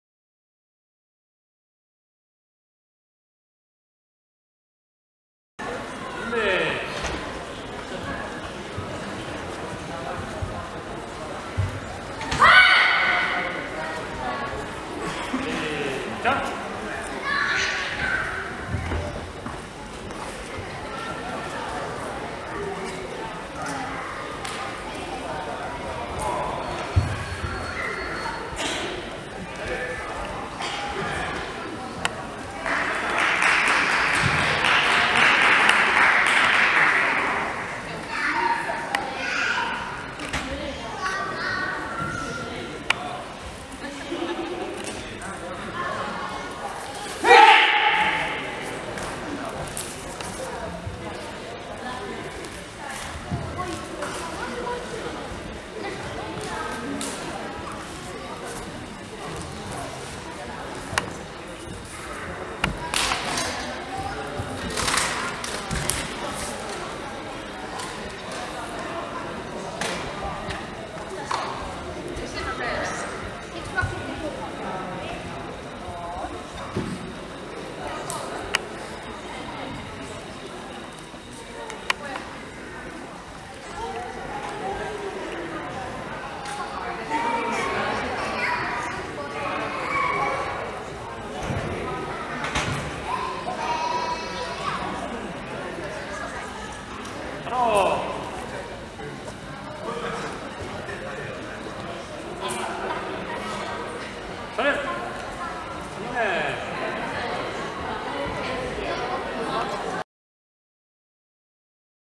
Henri Nordenswan & Kim Rejman & Ilja Patrikainen Lohja 1.3.2014 Chonkwon
Helsingitaekwondoseuran Henkka ja Kim sekä Mukwan Jyväskylän Ilja ("Tres amigos des seniors") olivat voittajia Lohja Openissa sarjassa ryhmät yli 30-vuotiaat miehet. Tämän suorituksen accuracy-pisteet olivat 2,63 ja presentation pisteet 3,43.